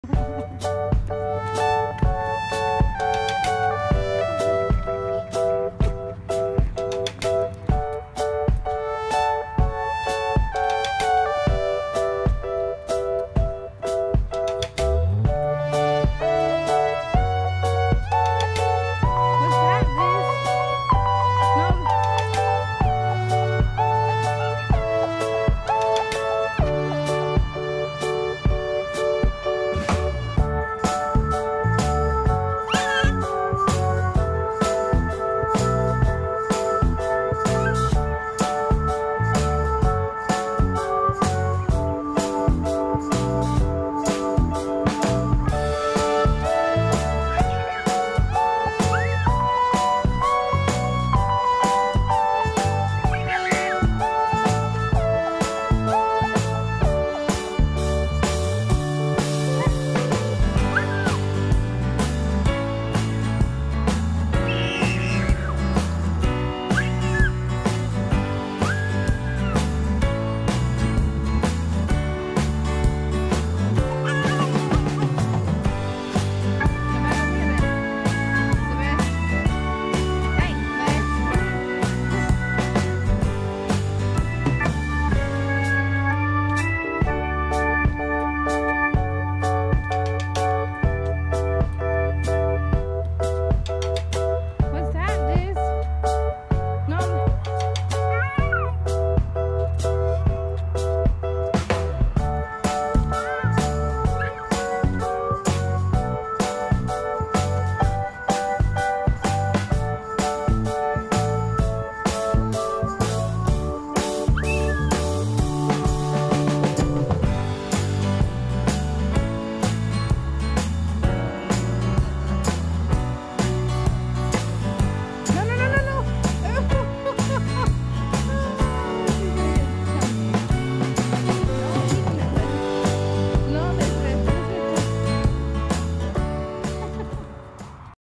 Learning new things daily activities for toddlers fun time with princess Elizabeth
Bubbles time at the park having a blast fun time with the kids